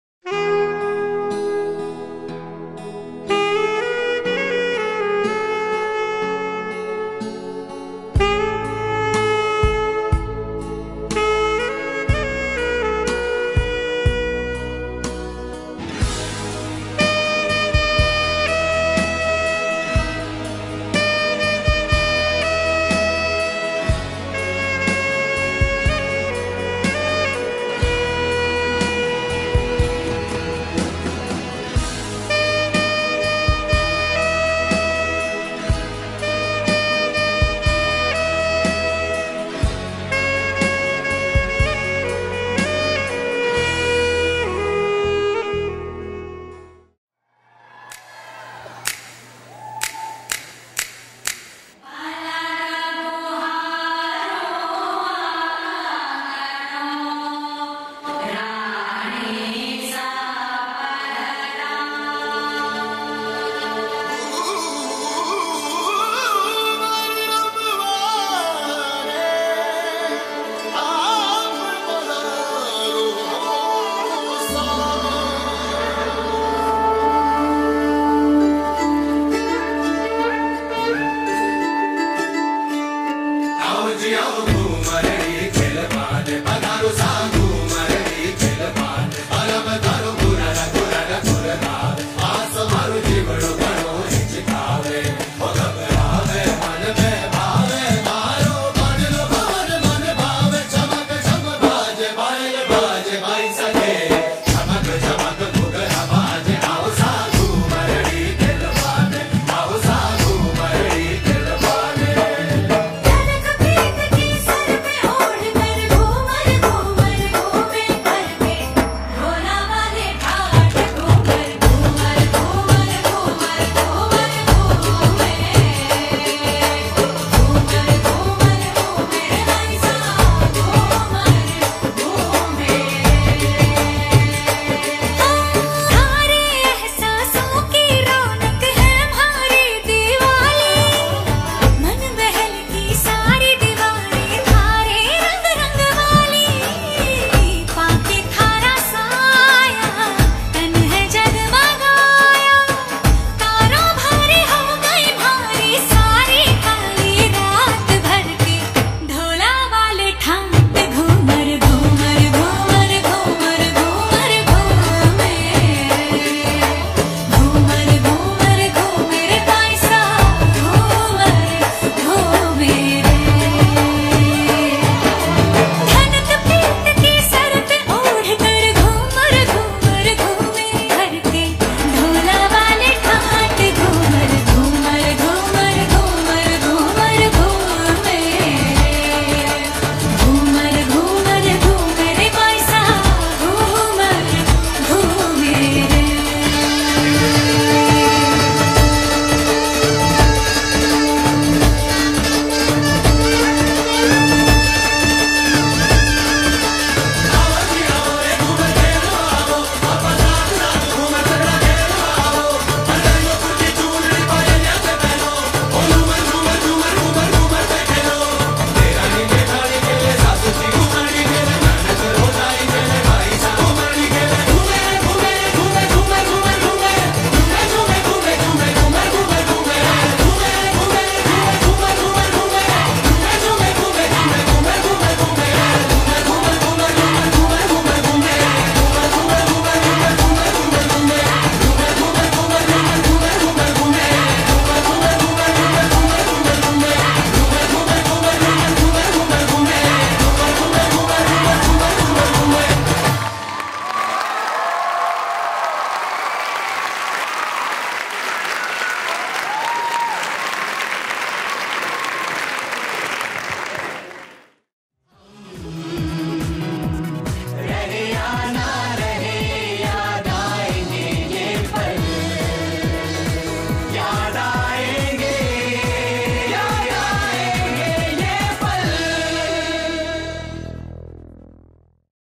Ghoomar Dance performance by Mayuri & Team @ Nache Mayuri's Pal, Annual Dance Show 2018
Ghoomar dance was performed by Mayuri  & team @ Nache Mayuri's Pal, the 5th Annual Dance Show held on May 18th 2018 at Lake Zurich Performing Arts Center.